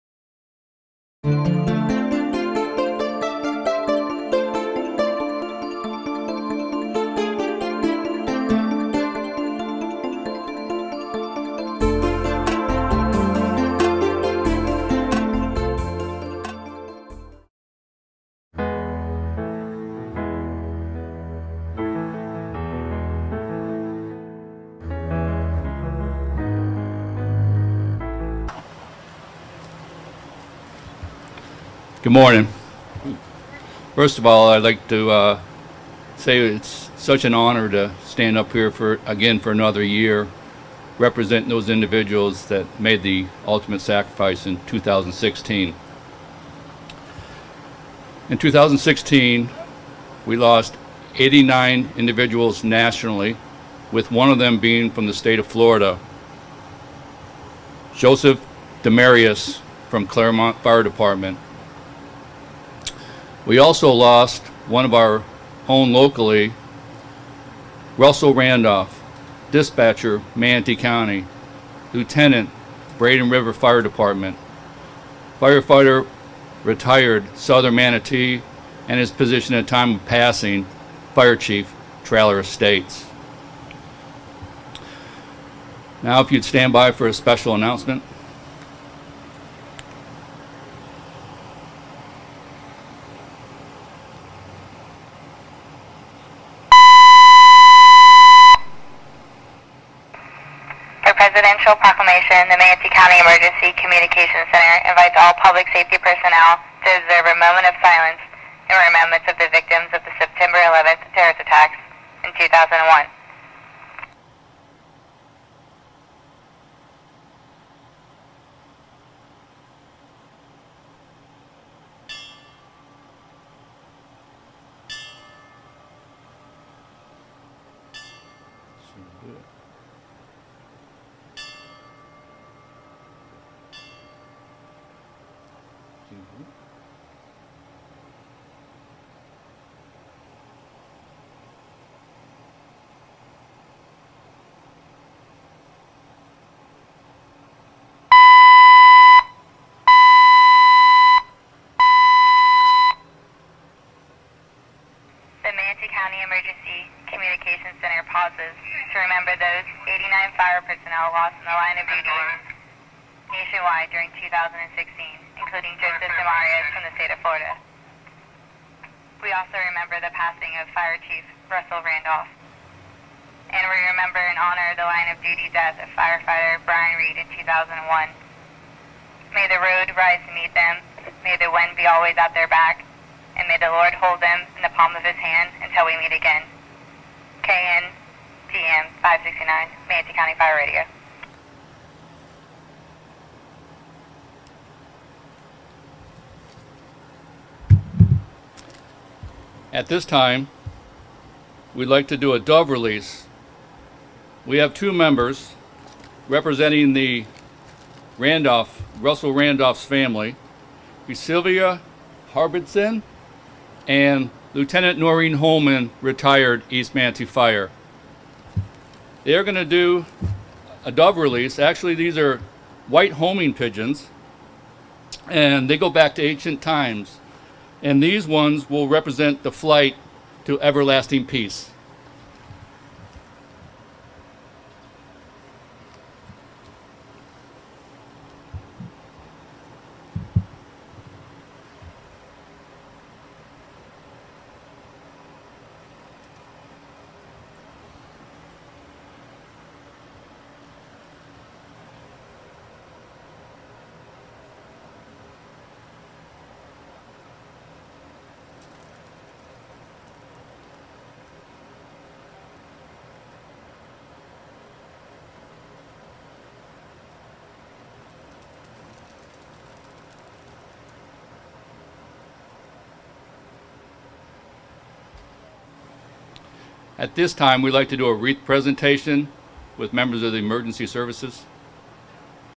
2017 Tribute to Heroes Manatee County
The event was held a month late due to Hurricane Irma.